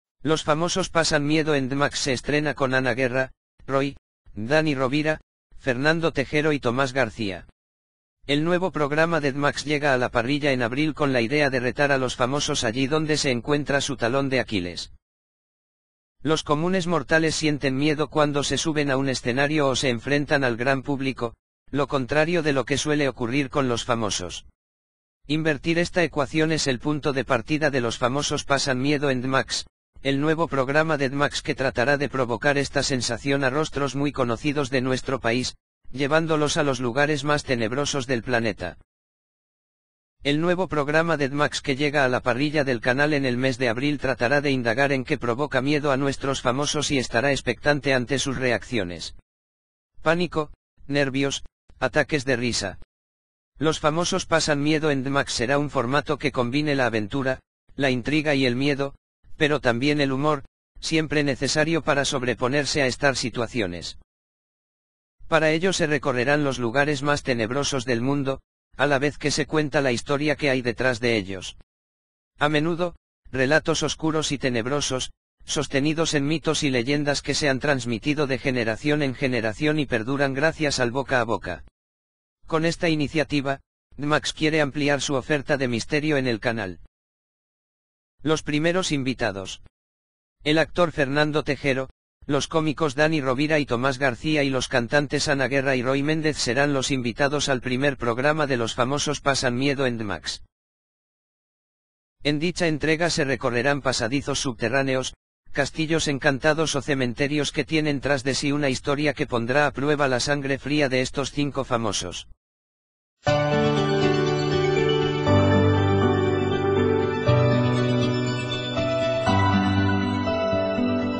'Los famosos pasan miedo en DMAX' se estrena con Ana Guerra, Roi, Dani Rovira
'Los famosos pasan miedo en DMAX' se estrena con Ana Guerra, Roi, Dani Rovira, Fernando Tejero y Tomás García

★ Summary ➜El nuevo programa de DMAX llega a la parrilla en abril con la idea de retar a los famosos allí donde se encuentra su talón de Aquiles.